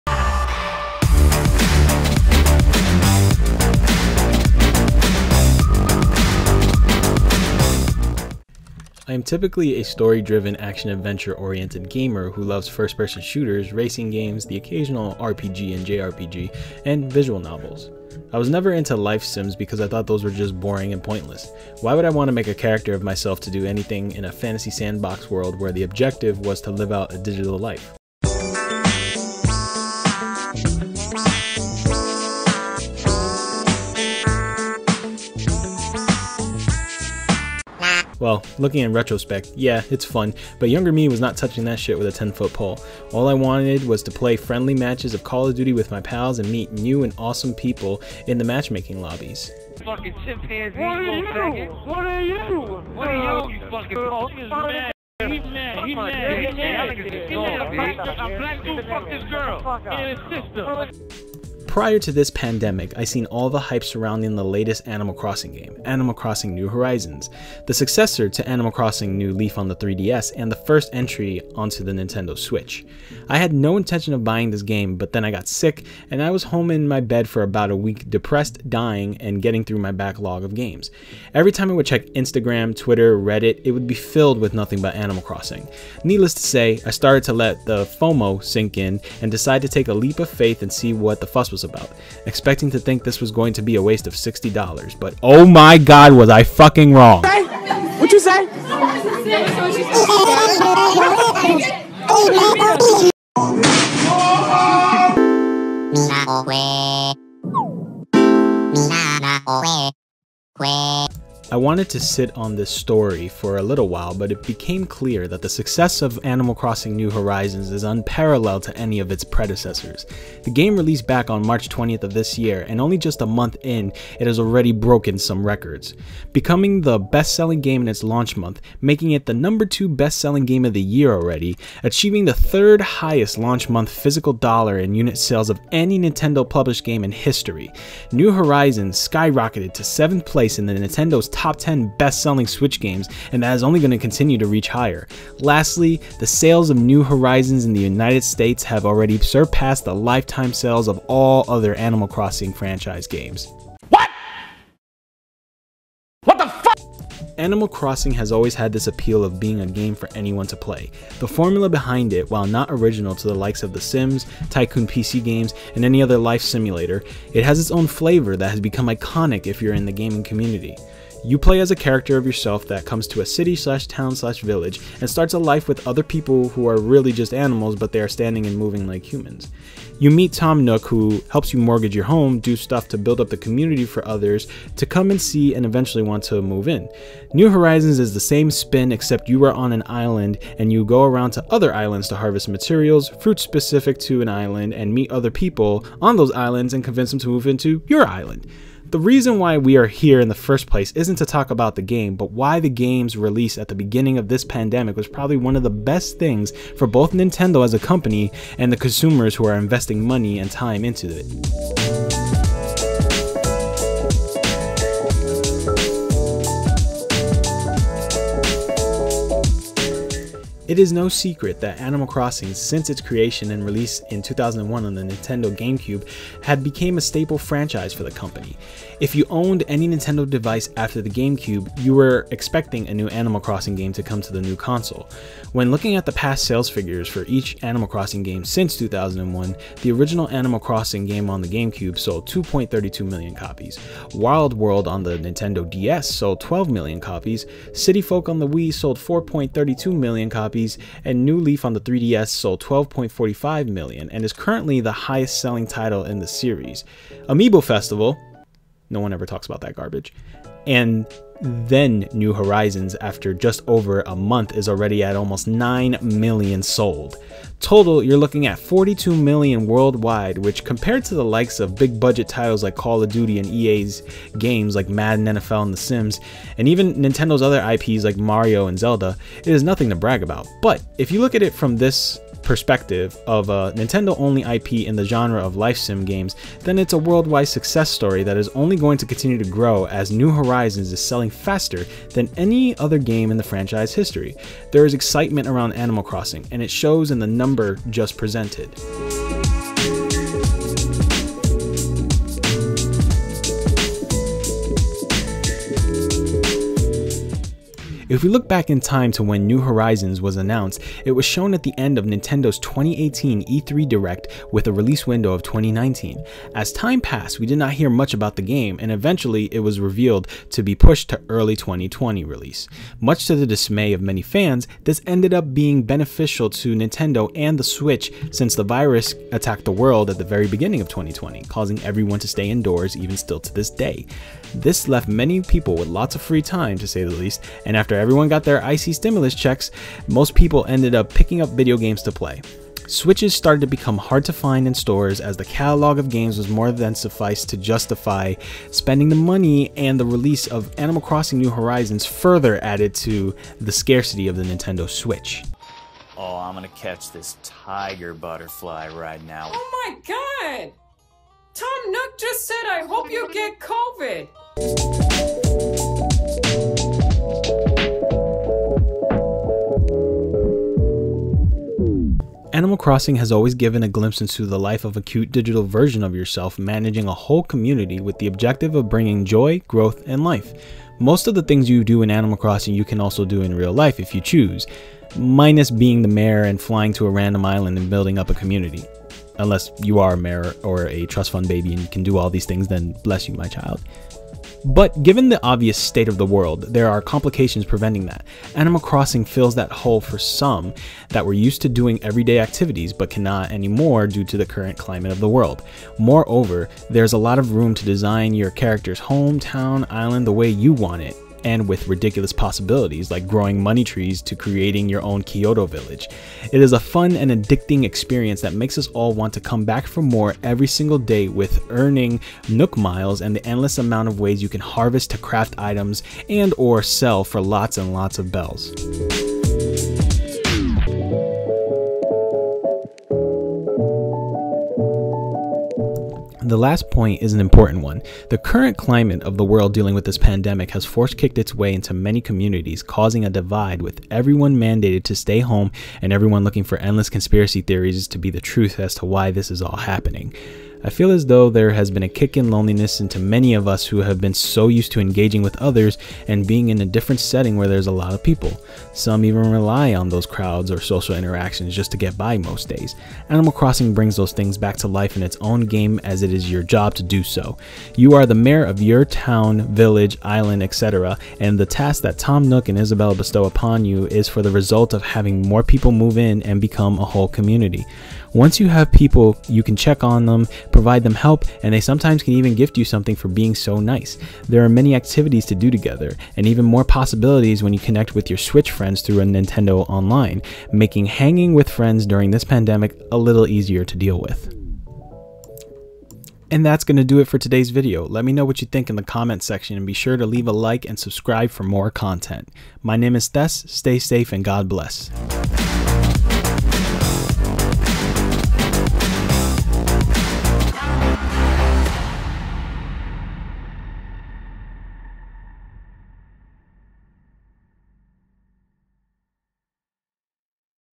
Animal Crossing: New Horizons & Why It's A Critical Success Now
Yo!

Thess here bringing you a new video today!

I talk about Animal Crossing: New Horizons and Why it is already a success story in 2020 even with the pandemic occurring.